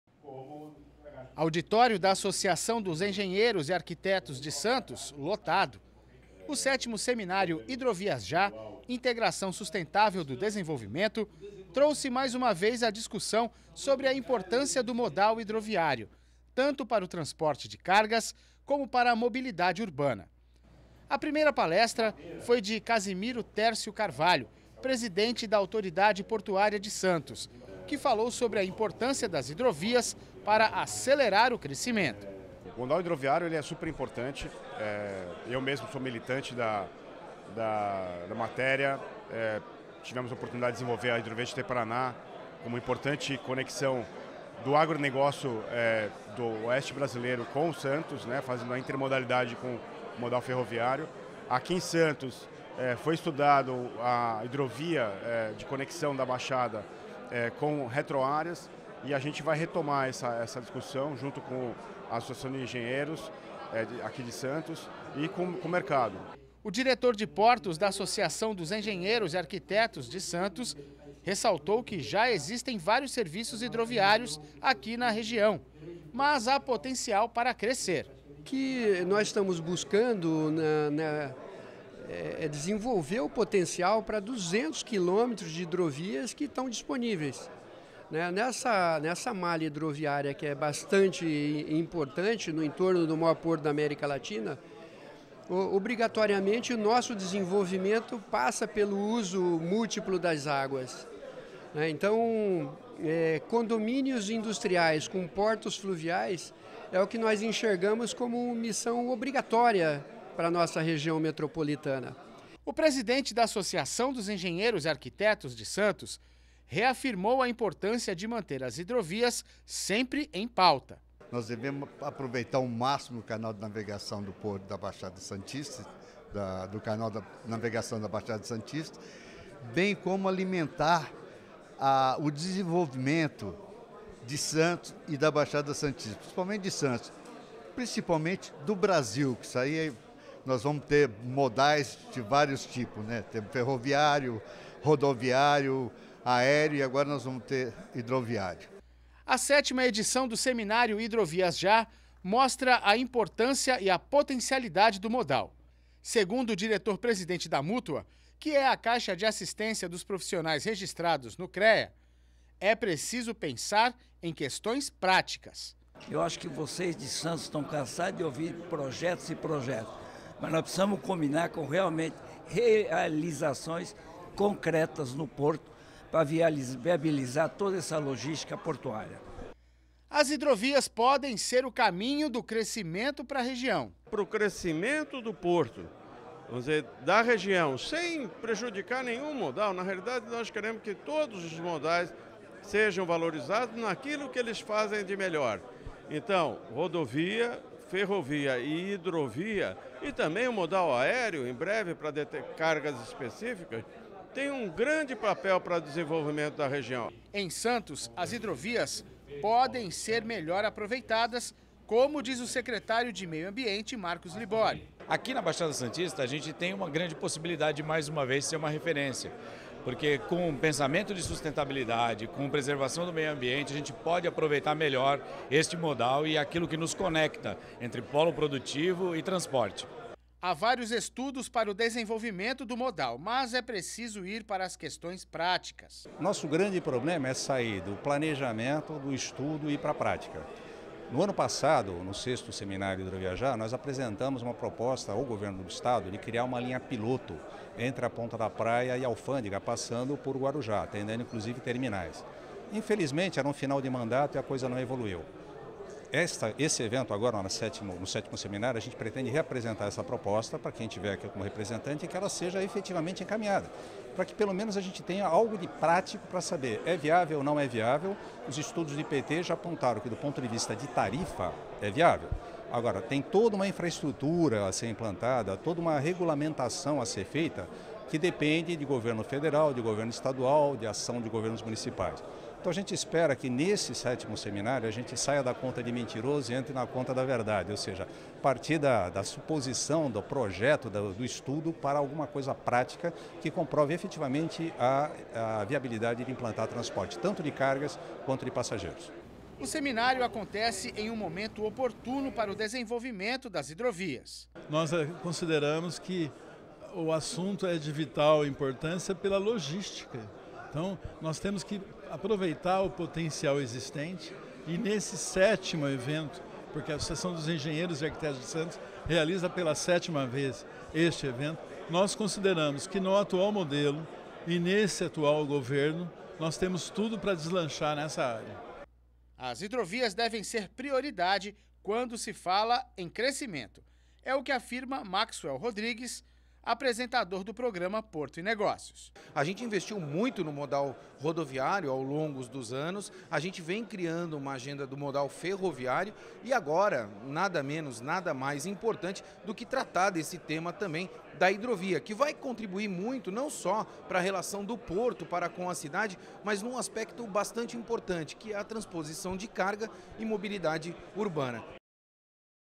A Associação de Engenheiros e Arquitetos de Santos realizou o 7º seminário de Hidrovias Já
A Associação de Engenheiros e Arquitetos de Santos realizou nesta terça-feira o sétimo seminário de Hidrovias Já, integração sustentável do desenvolvimento. O evento contou com especialistas e autoridades do setor e discutiu as melhores práticas para implementar o transporte hidroviário na região e no País